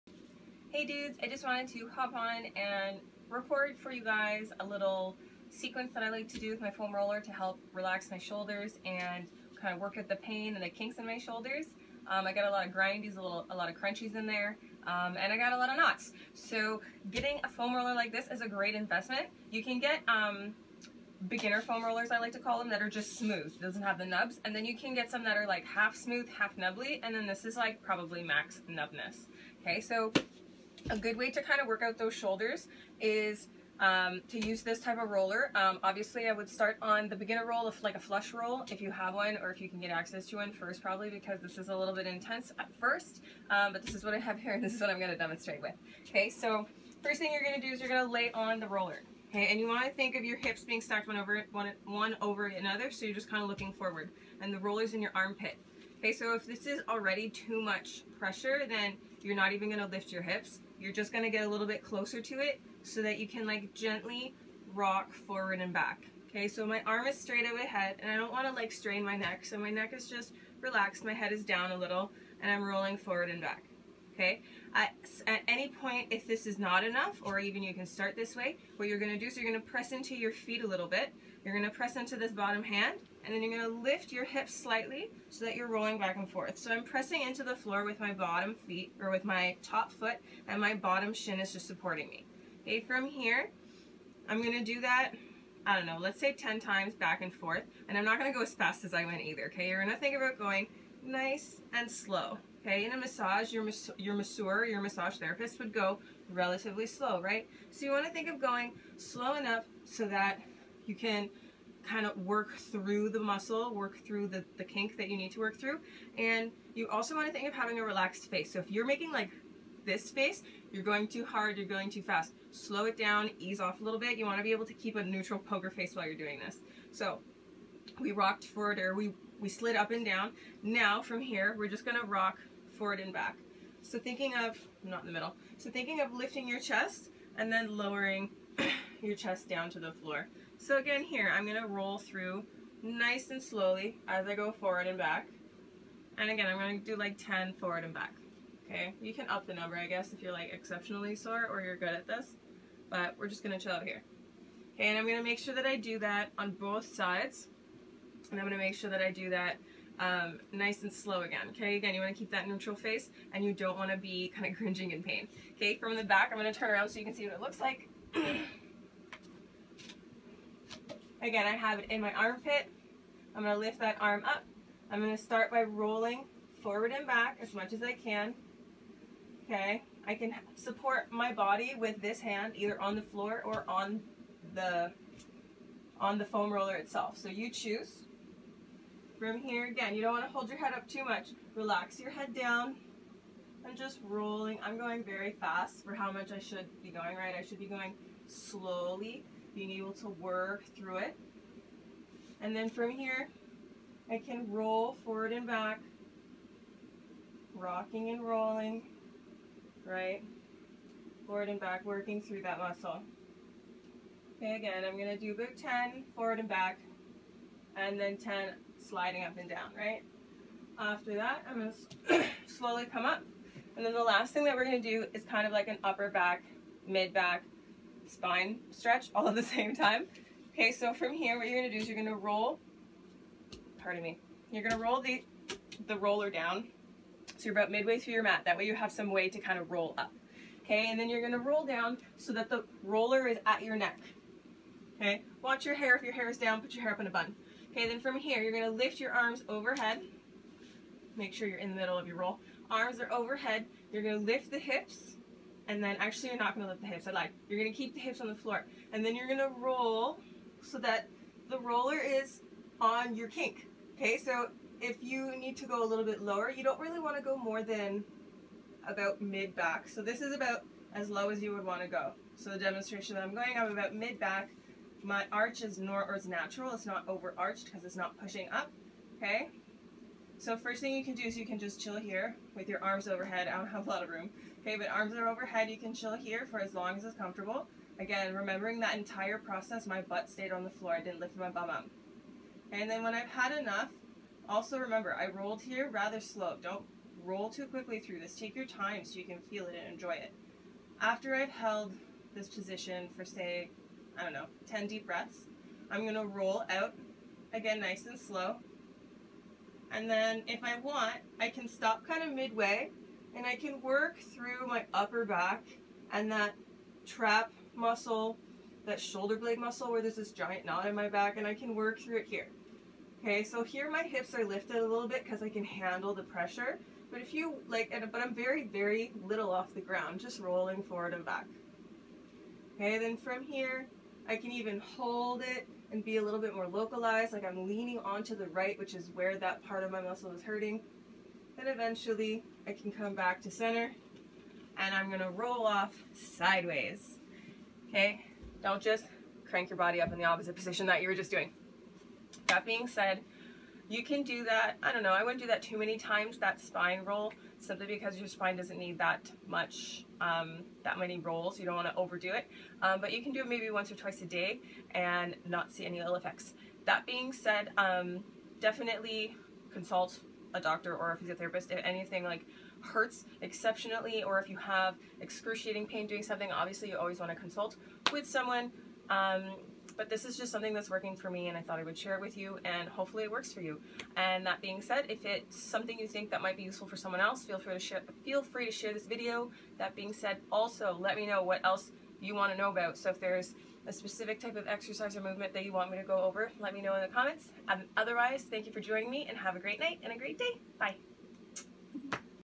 Sore Shoulder Self-Care
Hit the Rotator cuff, shoulder blades, and upper back, PLUS open the spine and chest! Improving back and spine flexibility, and reduce aches and pains.